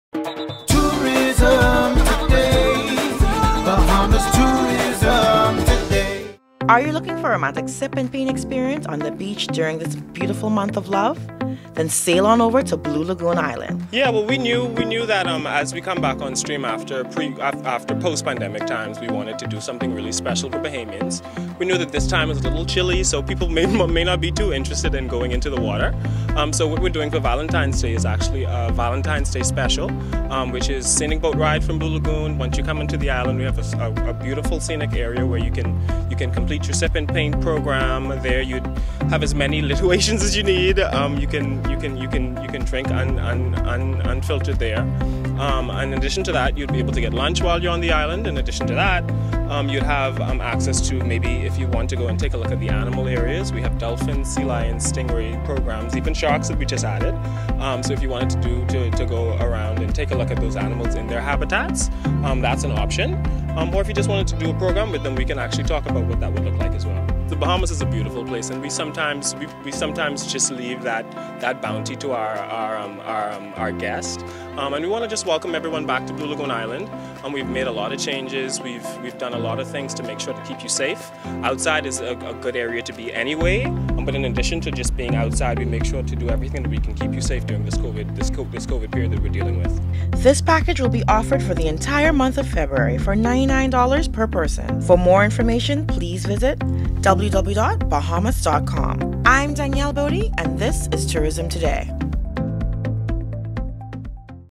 Tourism Today: Blue Lagoon Sip and Paint Experience
Looking for a romantic sip and paint experience or a scenic beach day with friends? Blue Lagoon Island offers this unique experience that allows guests to enjoy natural views, beautiful wildlife, and the option to simply relax and unwind.